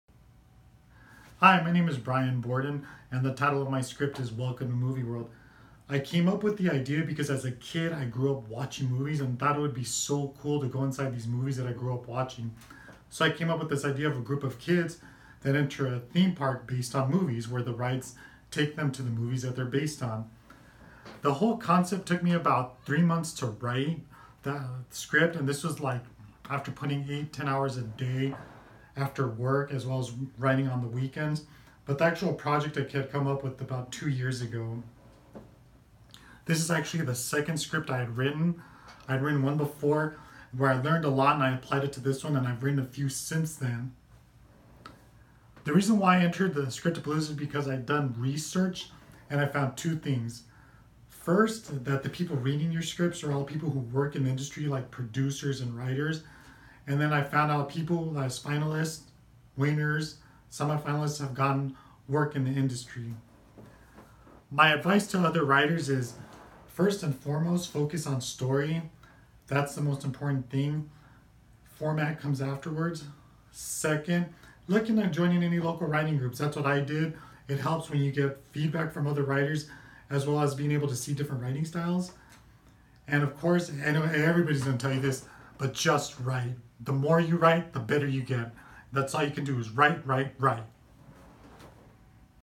2018 Scriptapalooza Screenplay Competition Runner-Up Bryan Bordon
2018 Scriptapalooza Screenplay Competition Runner-Up Bryan Bordon, he wrote WELCOME TO THE MOVIE WORLD.